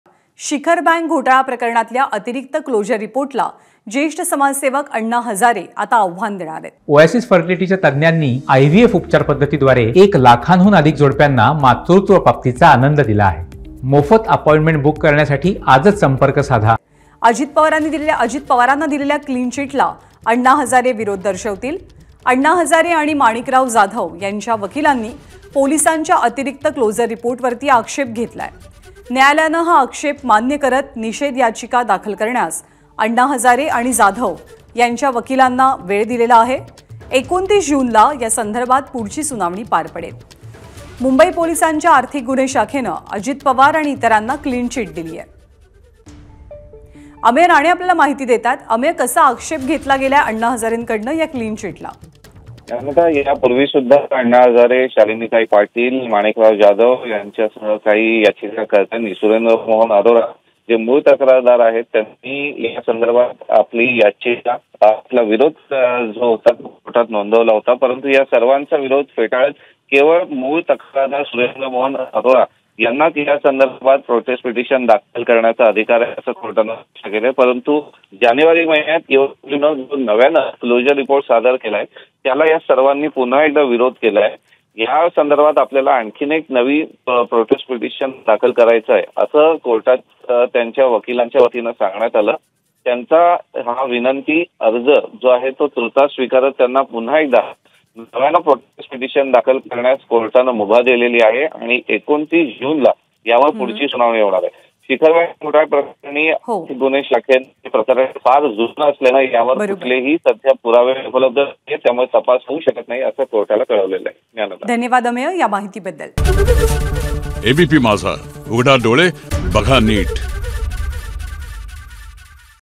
Anna Hazare : Ajit Pawar यांना दिलेल्या क्लिनचीटला अण्णा हजारेंचा विरोध
Anna Hazare : Ajit Pawar यांना दिलेल्या क्लिनचीटला अण्णा हजारेंचा विरोध, शिखर बँक क्लोजर रिपोर्टला आव्हान देणार